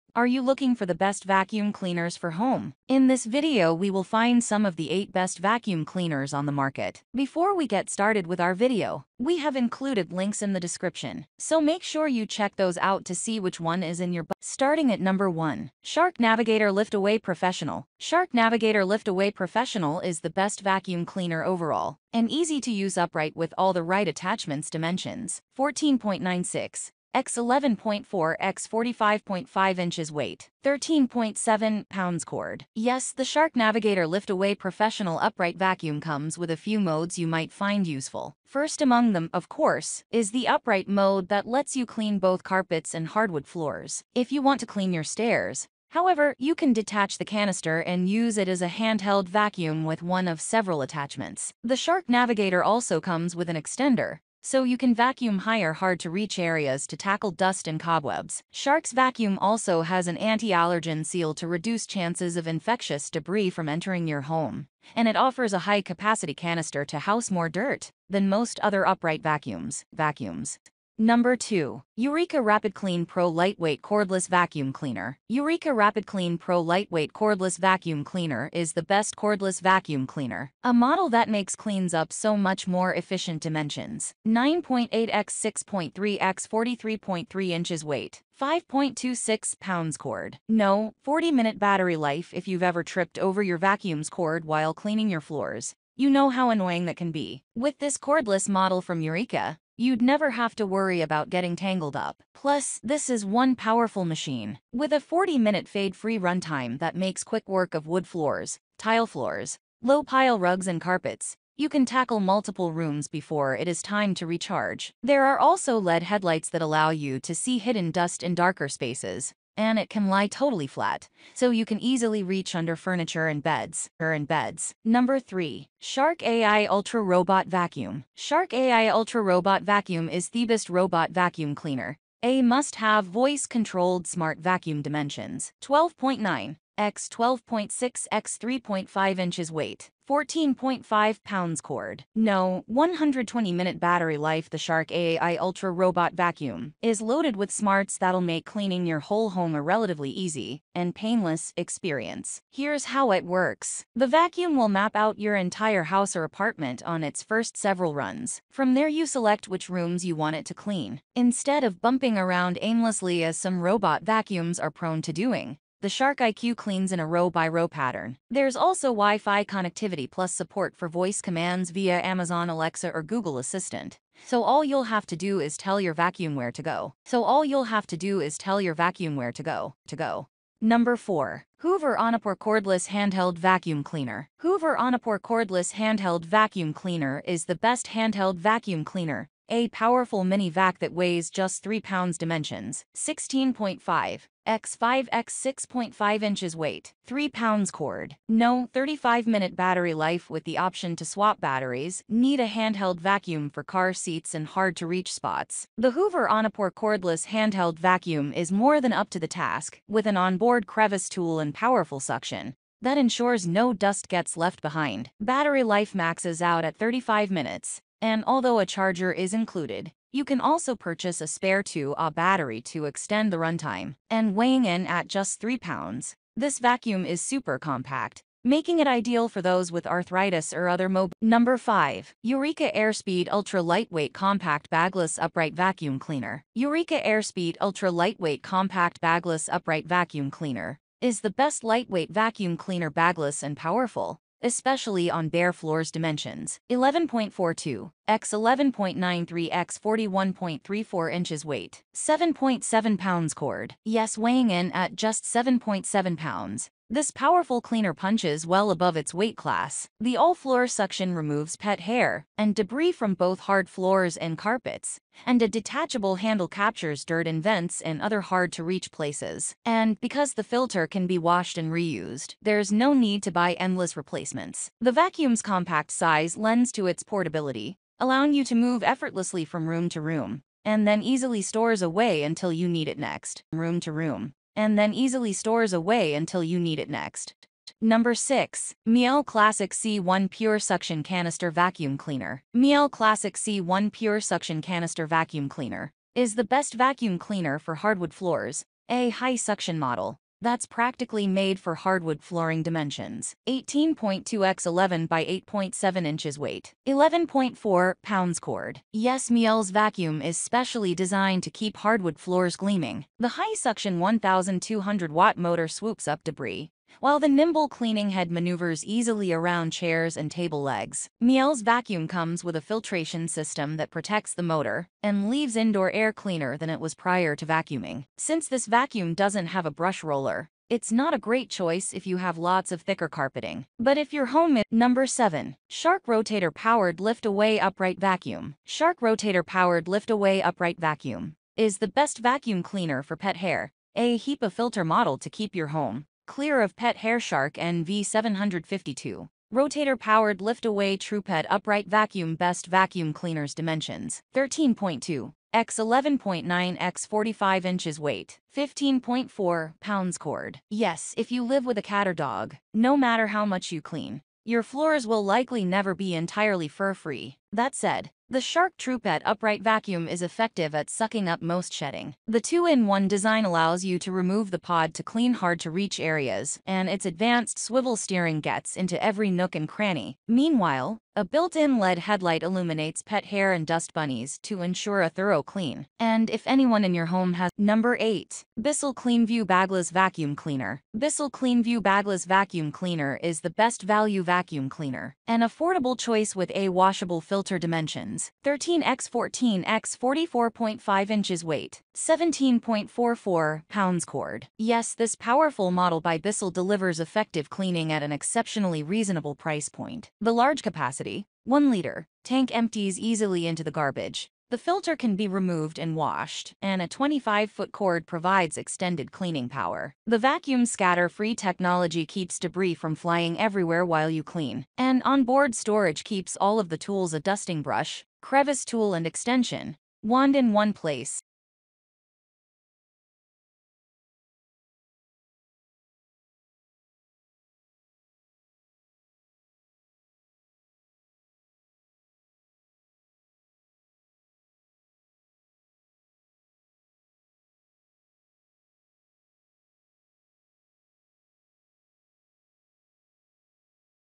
✅Best Vacuum Cleaners of 2023 |Top 8 : Best Vacuum Cleaners For home - buying guide
📌Product Link📌:
1. Shark Navigator Lift-Away Professional.

2. Eureka RapidClean Pro Lightweight Cordless Vacuum Cleaner.

3. Shark AI Ultra Robot Vacuum.

4. Hoover OnePwr Cordless Handheld Vacuum Cleaner.

5. Eureka Airspeed Ultra-Lightweight Compact Bagless Upright Vacuum Cleaner.

6. Miele Classic C1 Pure Suction Canister Vacuum Cleaner.

 7. Shark Rotator Powered Lift-Away Upright Vacuum.

8. BISSELL Cleanview Bagless Vacuum Cleaner.

In this video, we listed the Best Best Vacuum Cleaners.